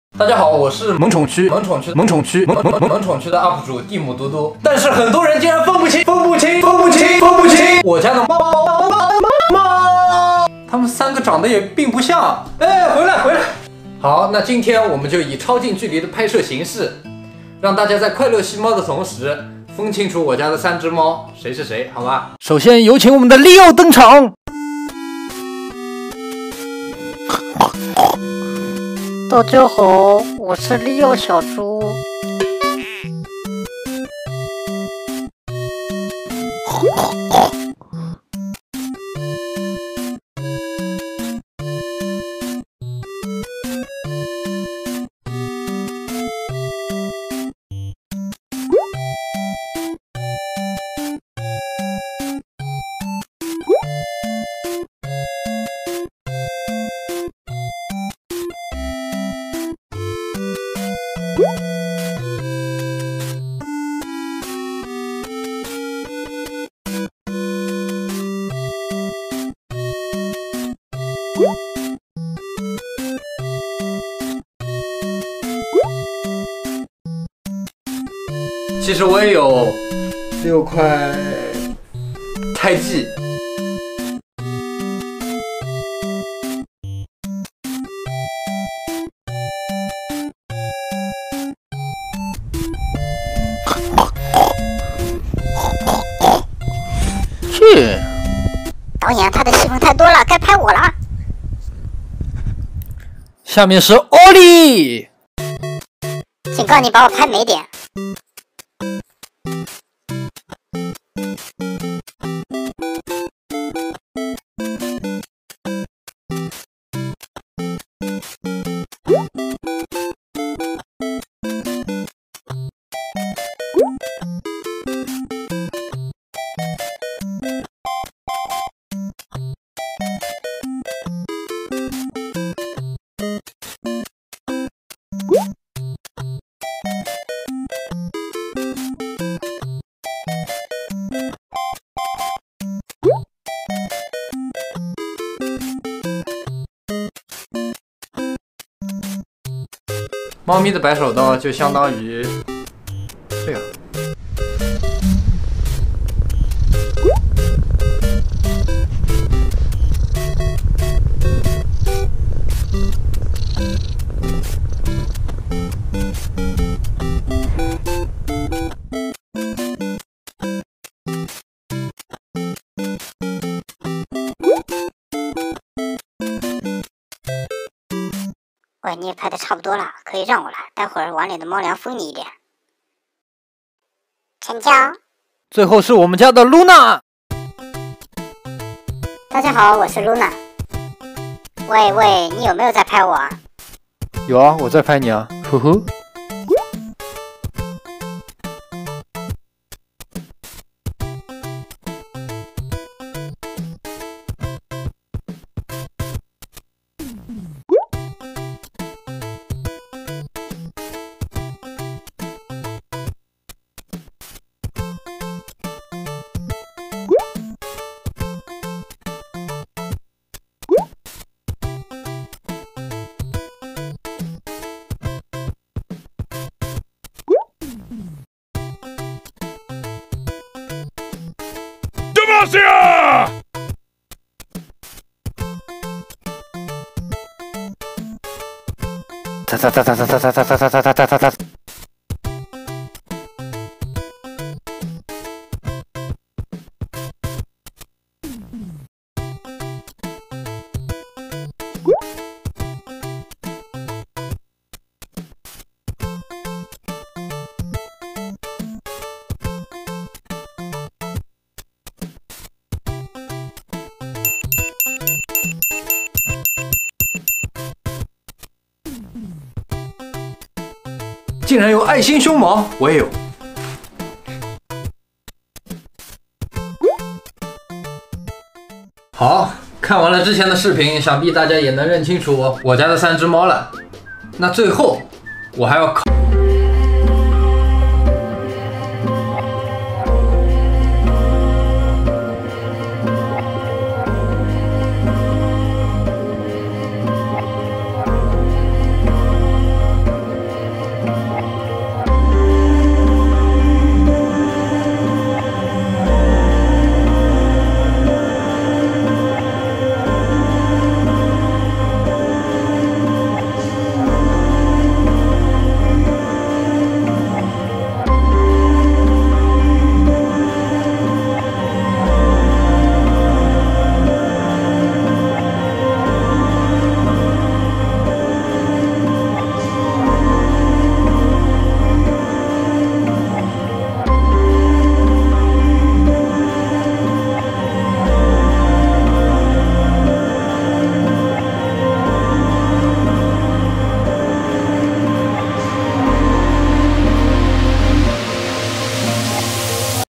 近距离云吸猫，带你了解我家猫咪的每一个部位
听说我是你的一见钟情喵？
盘点猫的各种可爱场面！万物皆有灵，猫的灵性让人温馨治愈